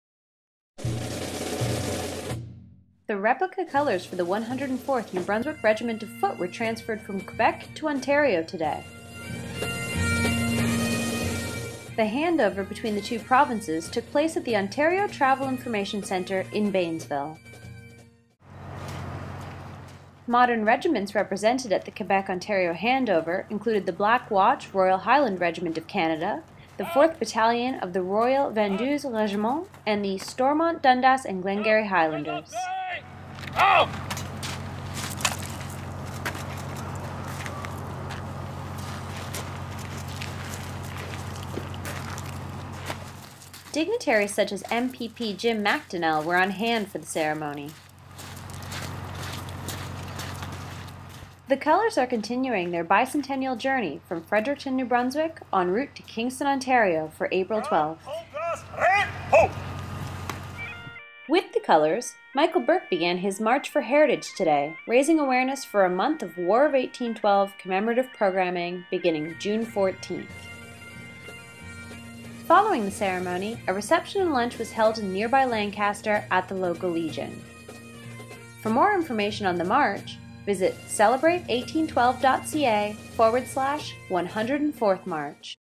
104th in Bainsville
The replica colours for the 104th (New Brunswick) Regiment of Foot moved from Quebec to Ontario today, April 6.